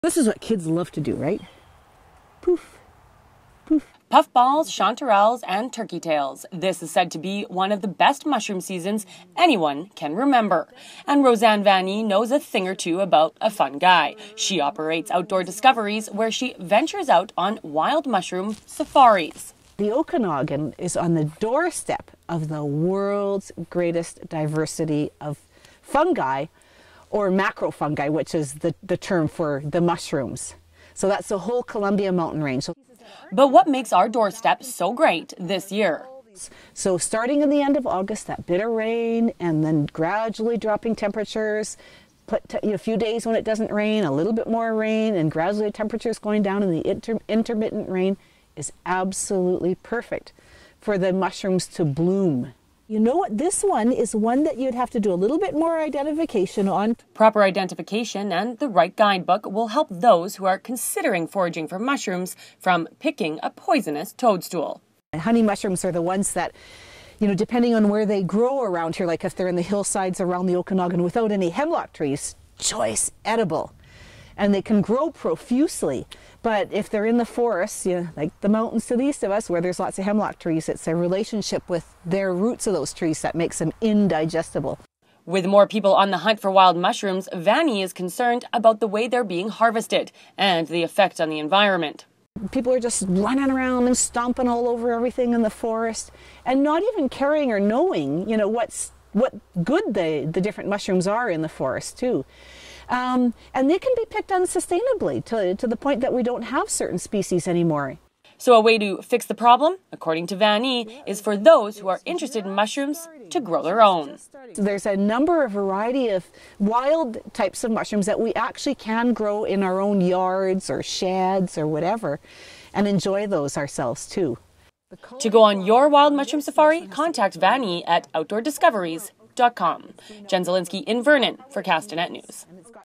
Feature: Best wild mushroom season ever in BC
Feature: Take a mushroom safari out in the Okanagan to discover this is the best wild mushroom season anyone has ever seen. (video: Jen Zielinski)